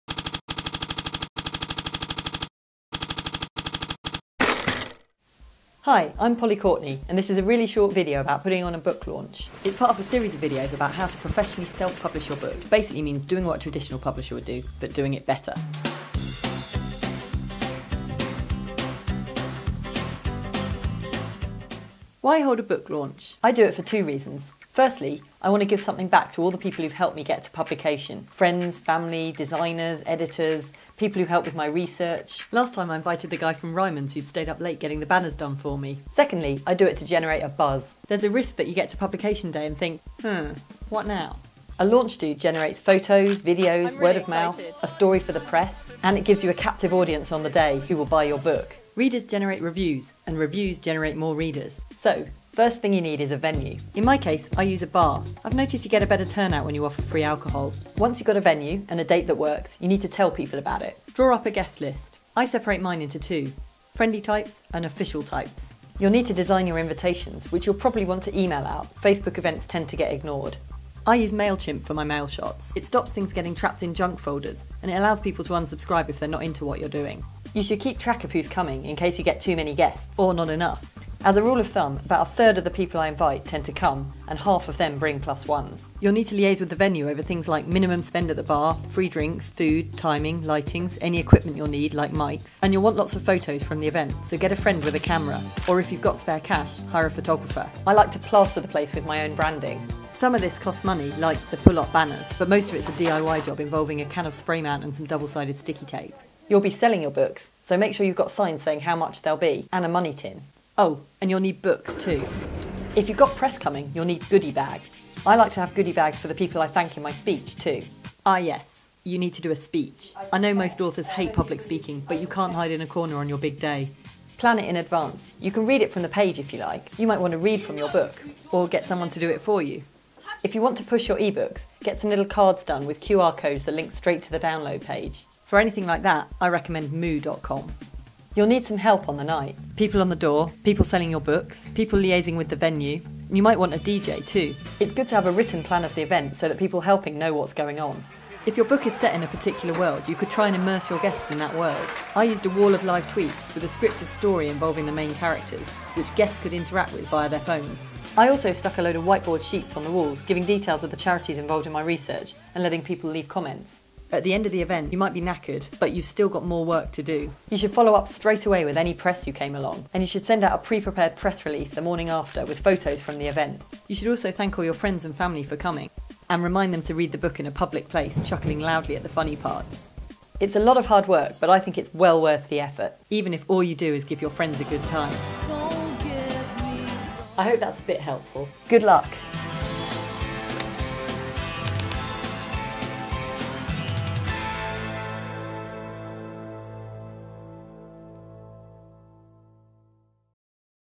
Part 6: Holding an Epic Book Launch (Self-Publishing Professionally)
Polly Courtney is the author of six novels, both self-published and traditionally published. In 2011, she walked out on her publisher, HarperCollins, in protest at the chick-lit branding of her books. She is a firm advocate of self-publishing, but only when it's done well.